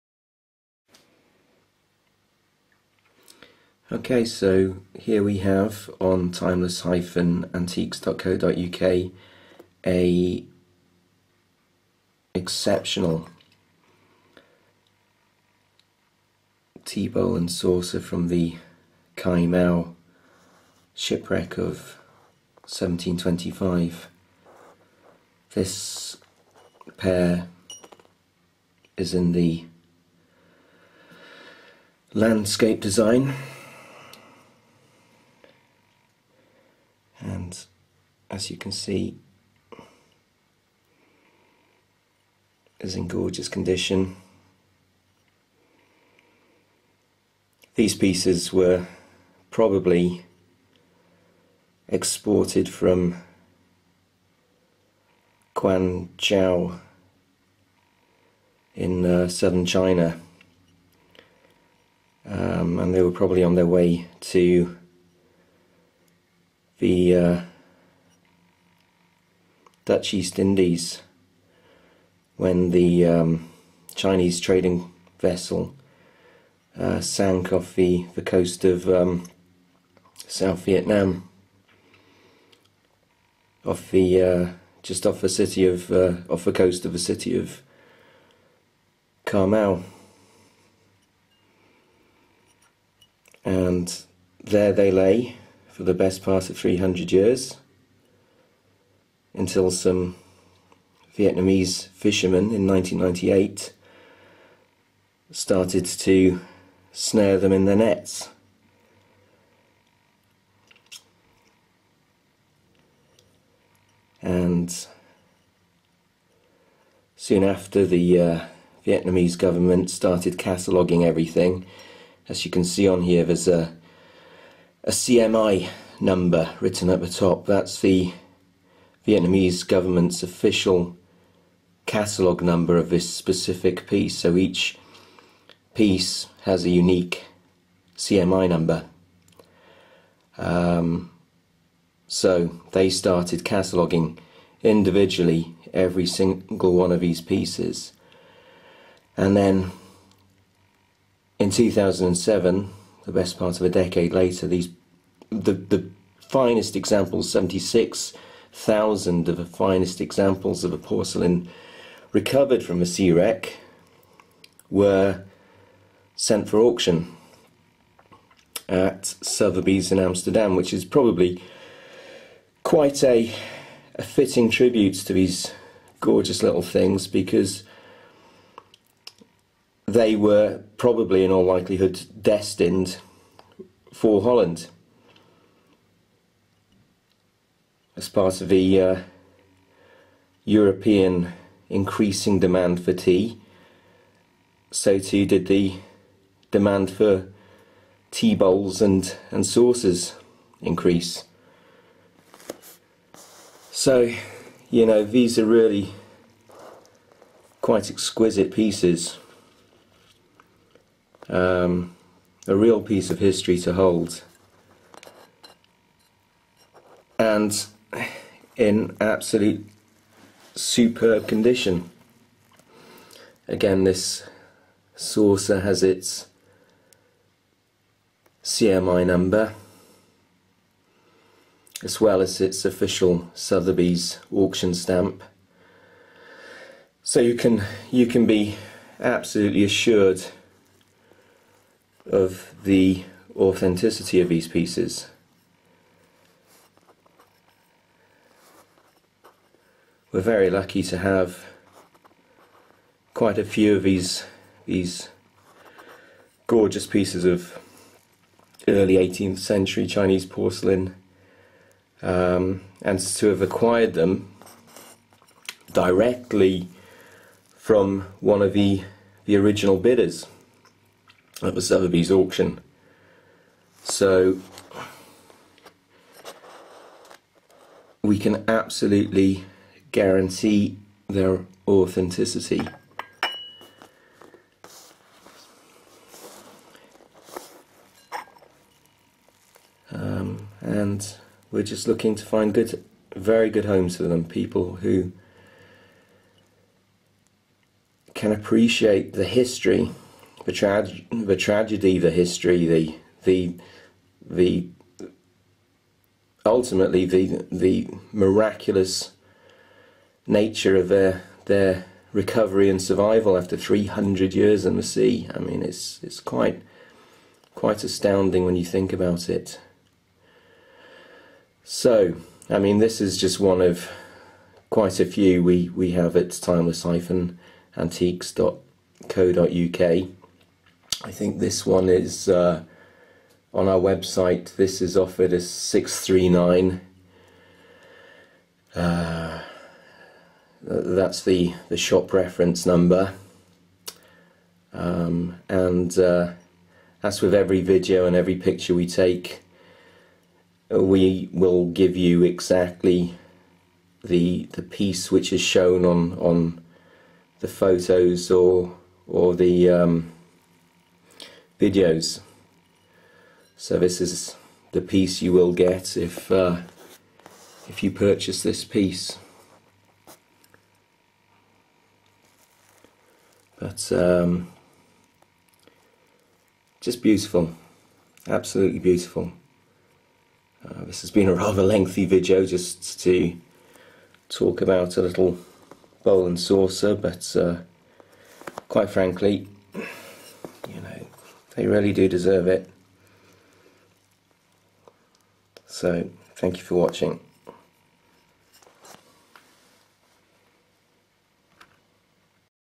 Ca Mau Shipwreck 'Landscape' Tea Bowl and Saucer - Circa 1725
This beautiful example of a tea bowl and saucer in the 'Landscape' design from the Sotheby's auctioned Ca Mau shipwreck is offered for sale exclusively